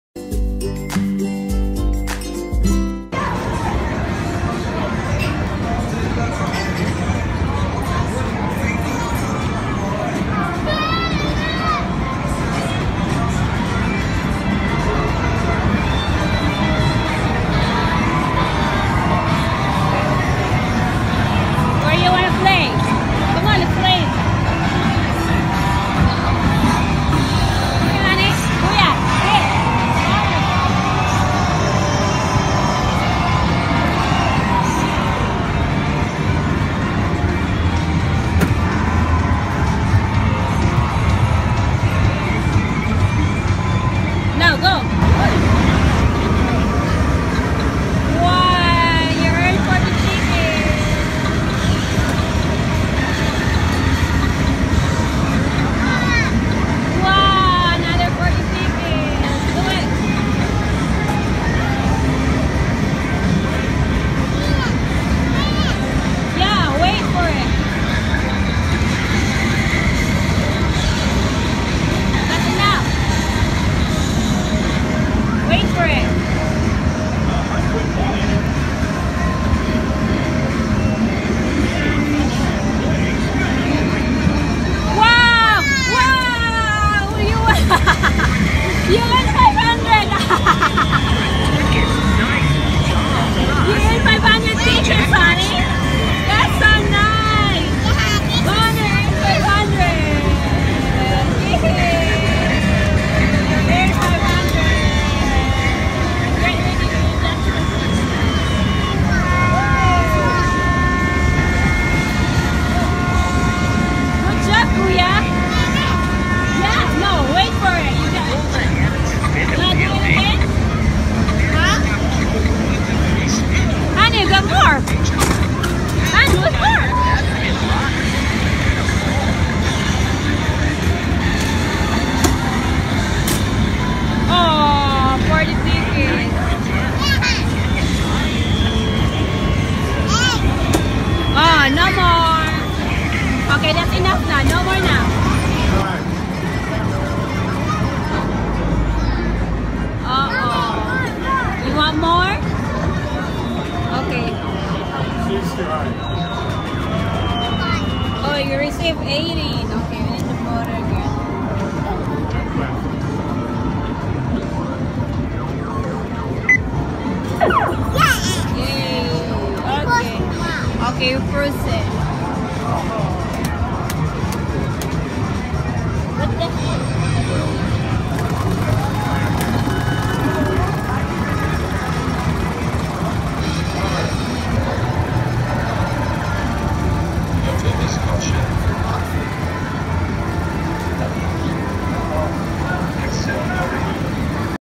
Birthday Boy won 500 Jackpot || Toddler Traveler
Celebrate my son’s 3rd Birthday at Dave and Buster and He won his first ever 500 Jackpot on his birthday, what a lucky kid